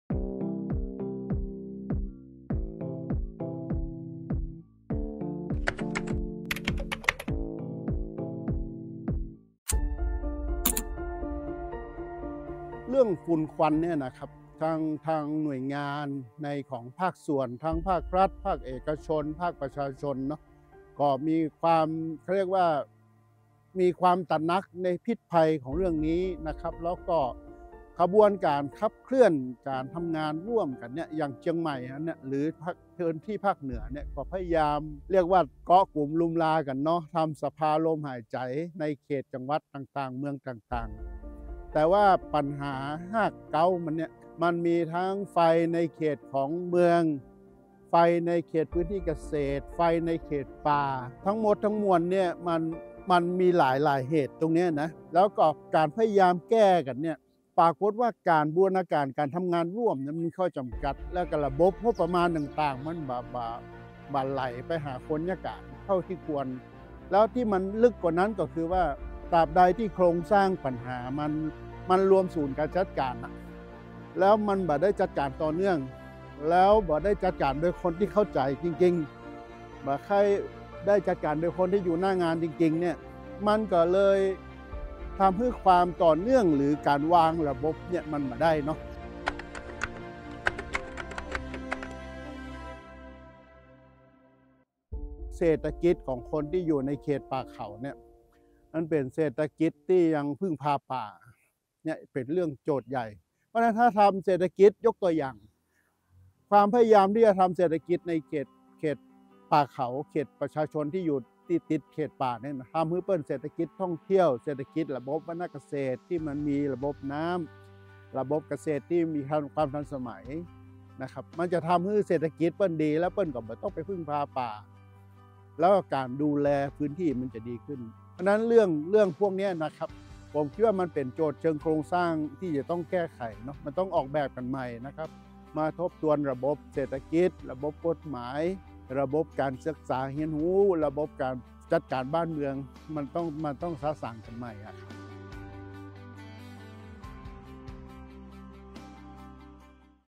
เลือกตั้ง 2566 : เสียงประชาชนถึงนายกรัฐมนตรีคนที่30 เสียง ดร.สมคิด แก้วทิพย์
ความหวังของคนไทย ที่ส่งถึงนายกรัฐมนตรี คนที่ 30 และรัฐบาลชุดใหม่ของประเทศไทย ชวนฟังเสียงของ ดร.สมคิด แก้วทิพย์ มหาวิทยาลัยแม่โจ้และหัวหน้าทีมวิจัยภาคเหนือ สกสว. ส่งเสียงถึงปัญหาฝุ่นควันในพื้นที่ภาคเหนือ ต้นตอและปัญหาเชิงโครงสร้างที่ส่งเสียงให้เป็นการบ้านไปยังรัฐบาลชุดใหม่ หลังเลือกตั้ง ปี 66